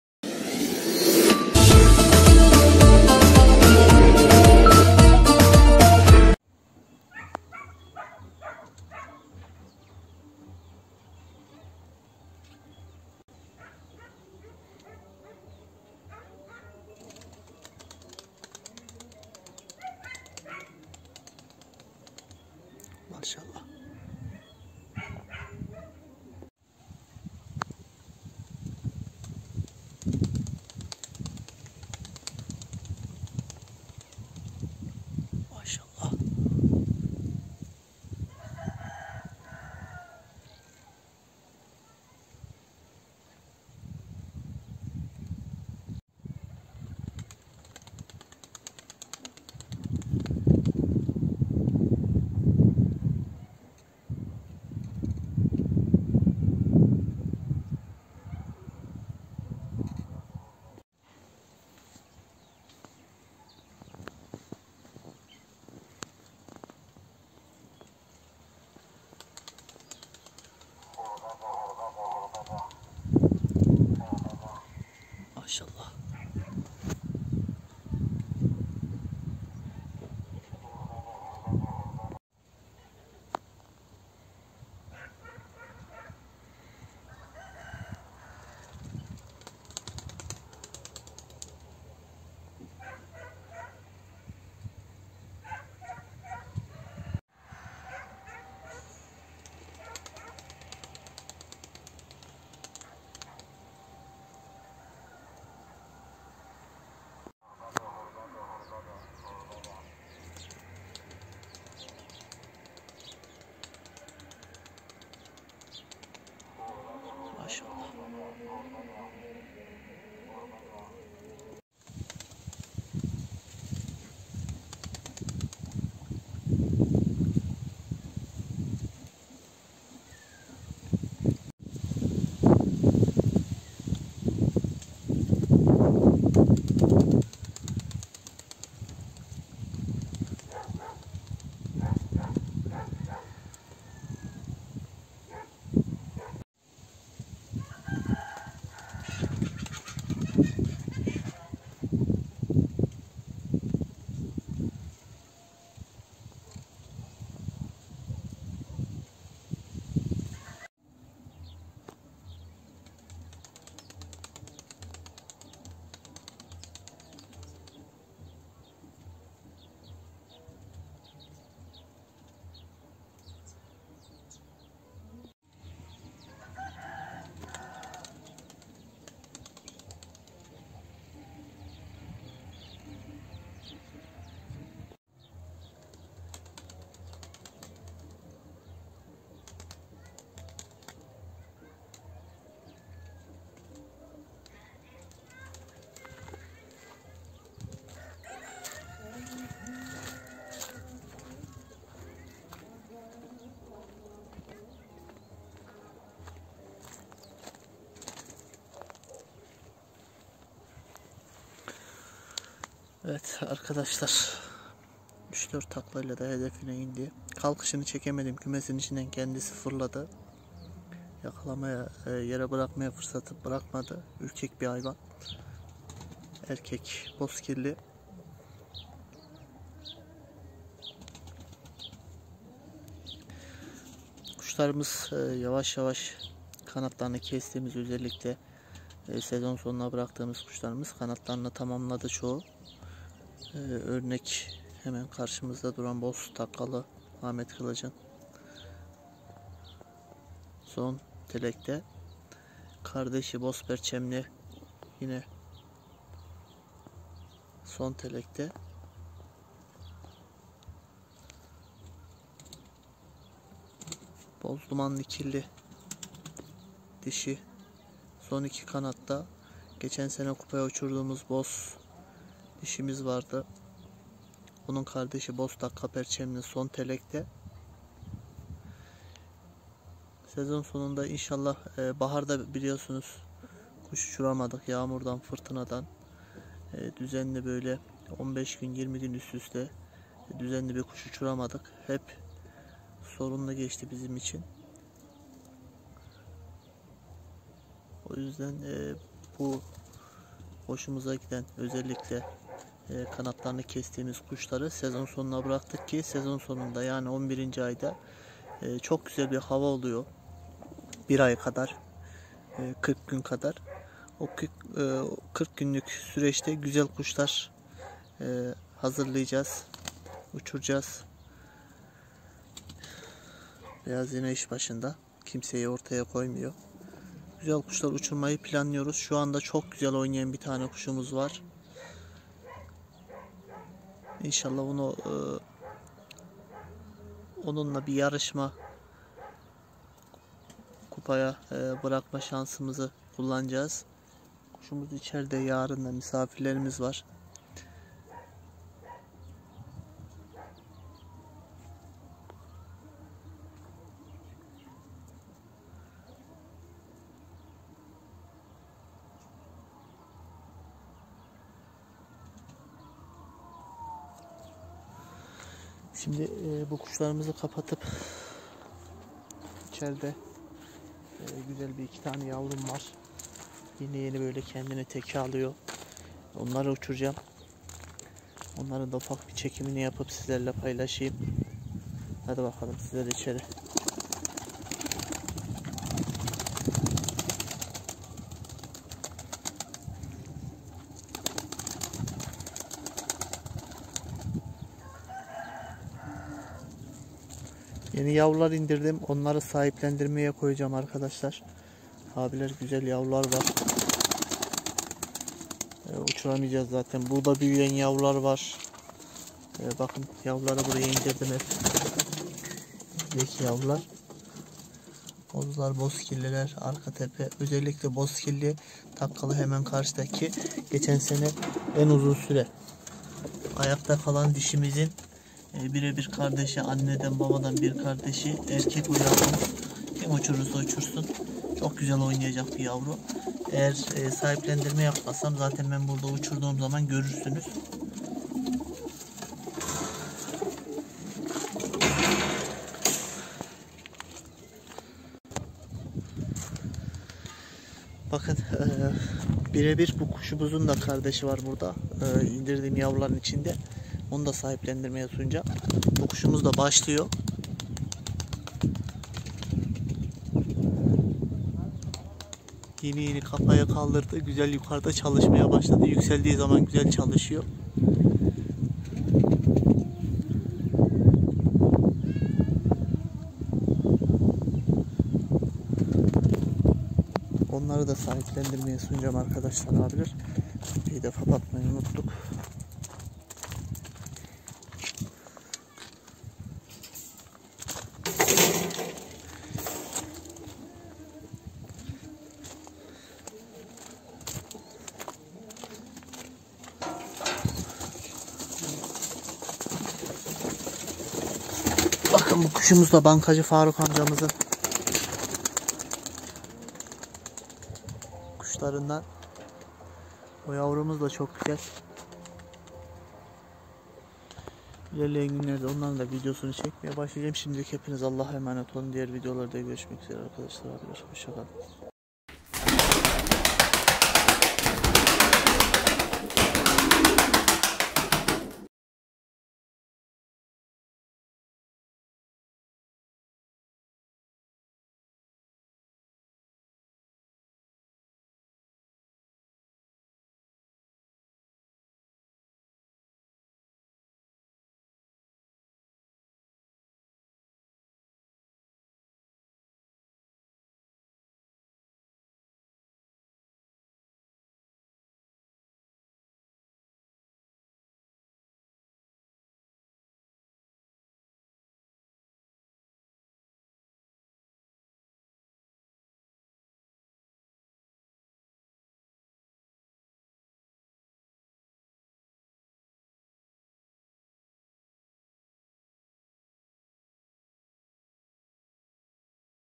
👍YARİN KUPAYA UÇACAK KUŞUMUZ👍 BÜYÜK ANKARA DOSTLUK KUPASİ NEJDET YILDIRIM CANLI TAKİP EDEBİLİRSİNİZ👍
SELAM aleyküm arkadaşlar hepinize teşekkür ederim kanalıma vermis oldugunuz desteklerden dolayı Allah hepinizden razı olsun kusumuz görüldüğü gibi anlatmaya gerek yok insallah beğenerek izlersiniz hepinizi Allah a emanet ediyorum dostlar..